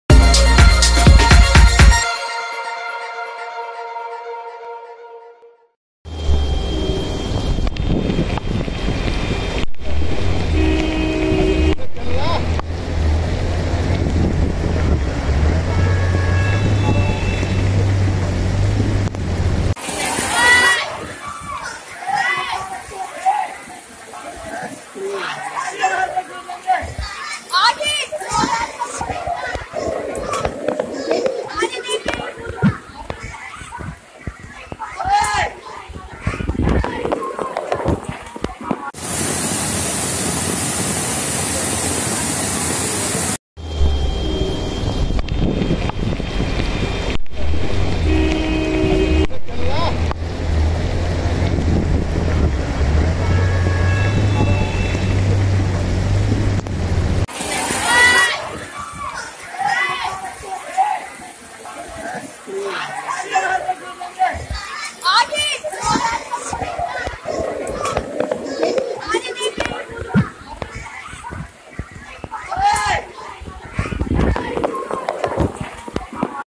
दिल्ली, यूपी कई जिलों में मूसलाधार बारिश
HEAVY RAIN DISTURBED NORMAL LIFE IN DELHI